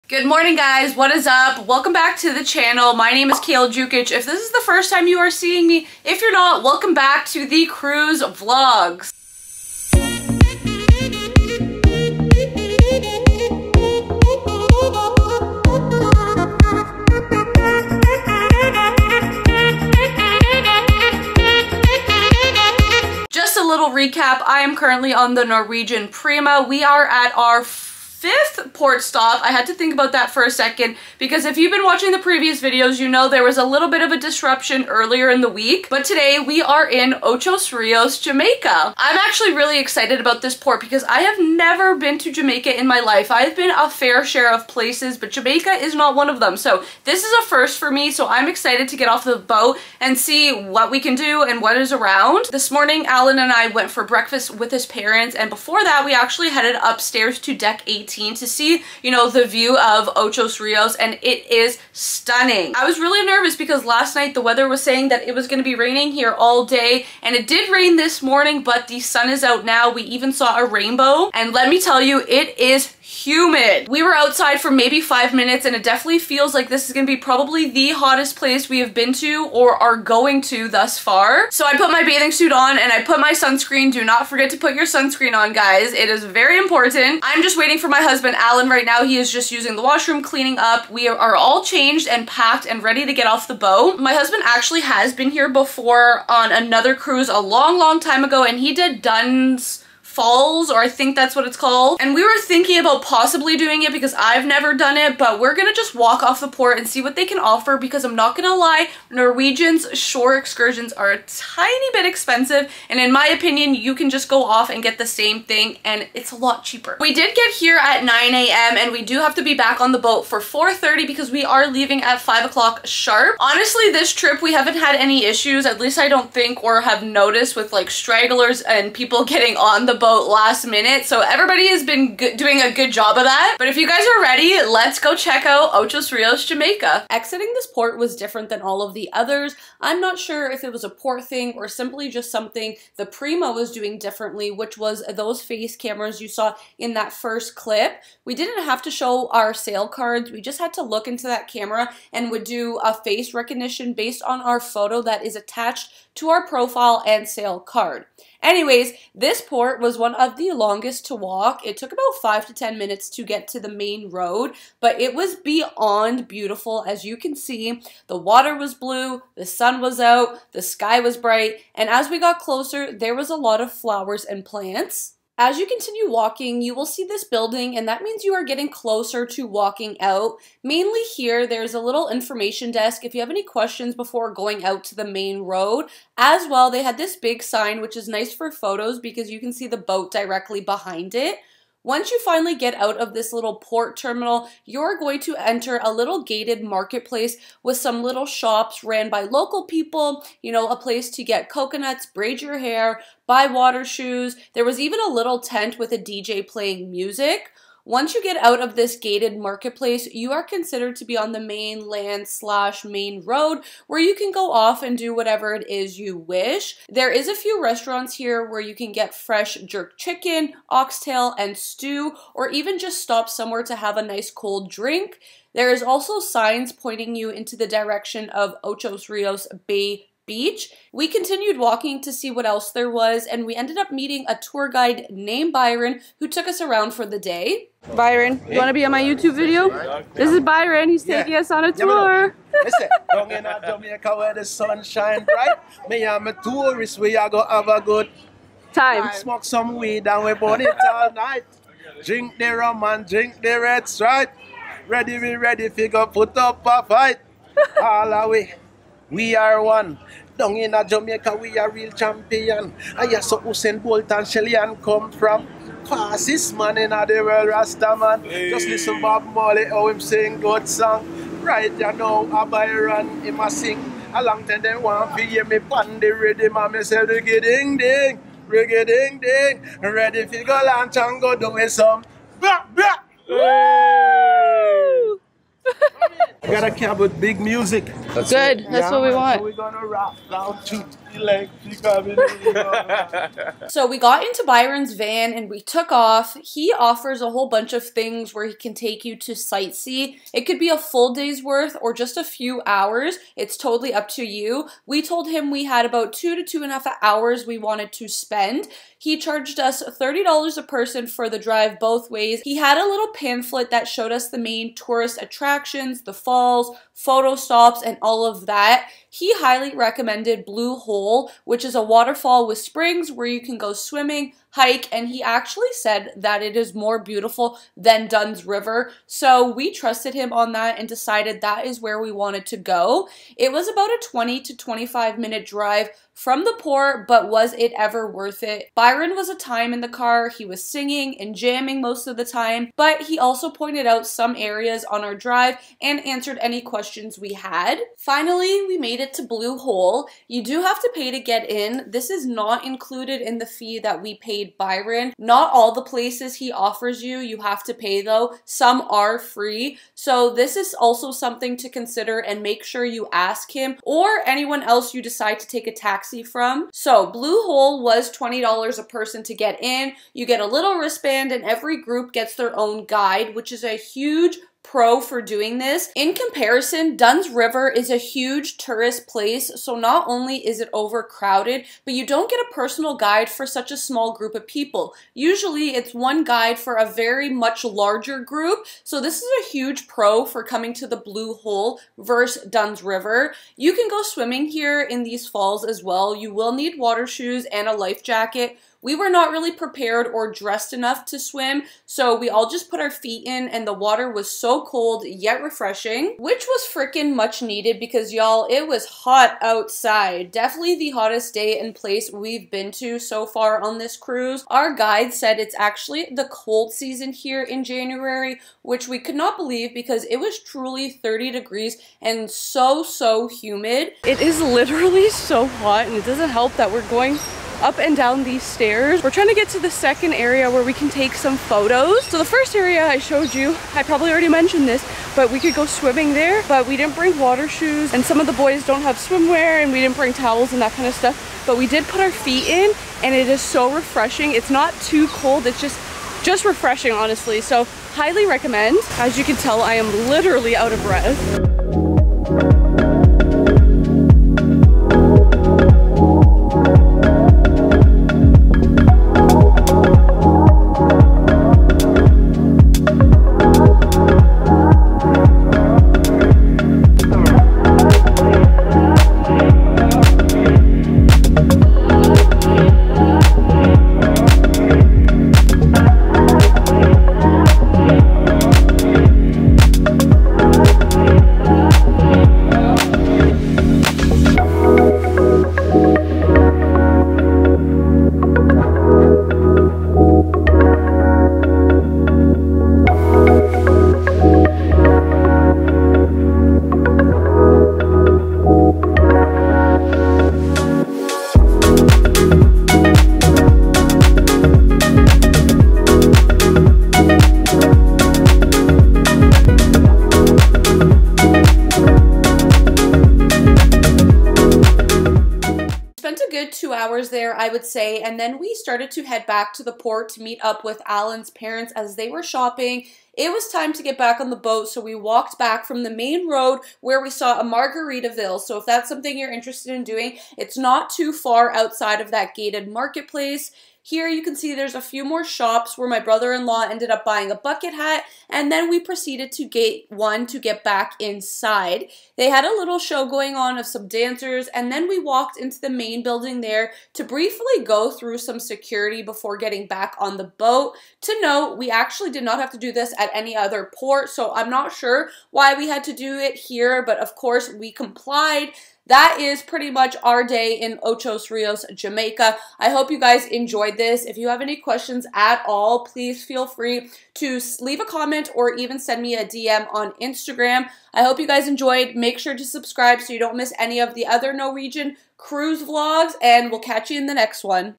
NORWEGIAN PRIMA 2023 | Ochos Rios, Jamaica cruise port + Blue Hole Jamaica 🇯🇲
welcome to my cruise series on the Norwegian Prima for the 9 day New Years cruise (from Friday December 30th, 2022 to Sunday January 8th, 2023). In this video I take you through the cruise port and our time at Blue Hole Jamaica (a must see!!). ENJOY (:

0:00 intro
0:21 today's agenda
1:14 tender boat
3:05 port terminal
4:27 main road/city
4:47 bayshore mall
6:10 photo stop
6:29 drive to turtle center
6:59 turtle center excursion
9:59 closing